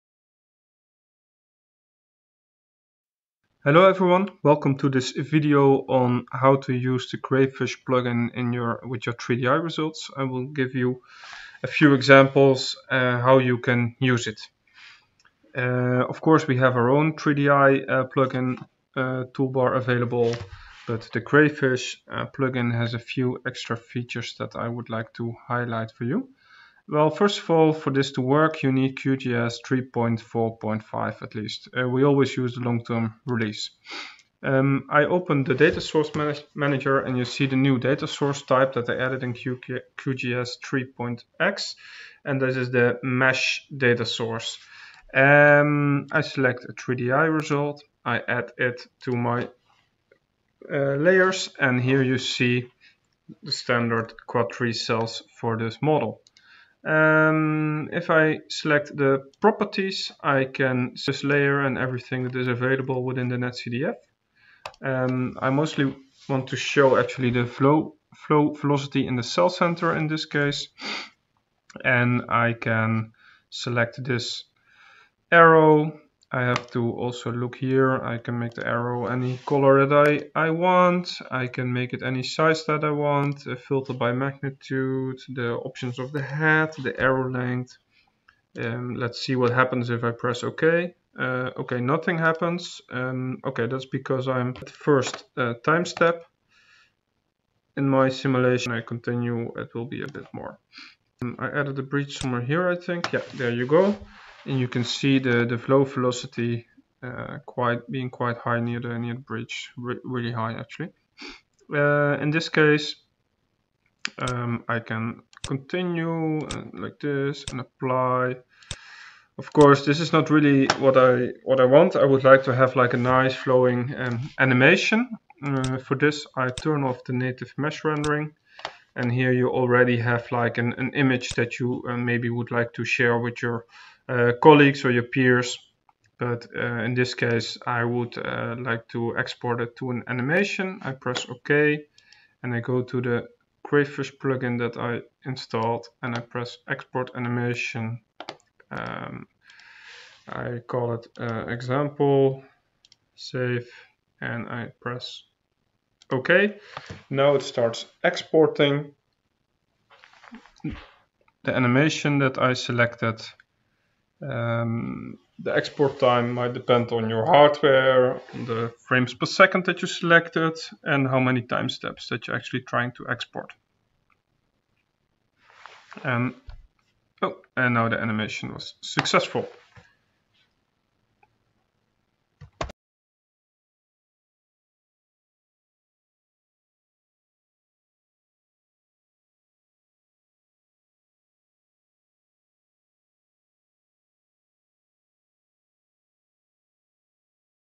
3Di Crayfish Plugin | Flow Velocity Animation
In this video we take 3Di results to create a flow velocity animation using the Crayfish plugin for QGIS.